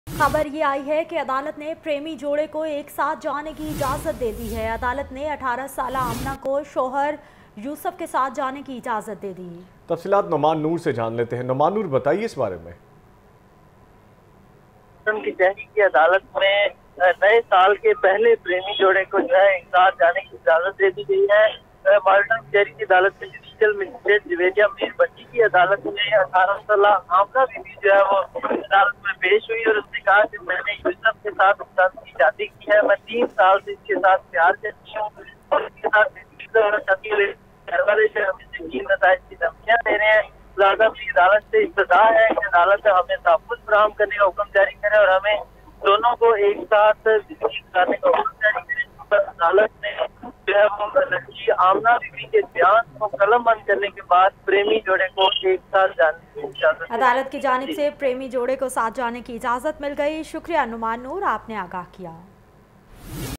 Court allows 18-years old Amna to live with Yusuf who she married without parent's consent
Court allows 18-years old Amna to stay with boy she married without parent's consent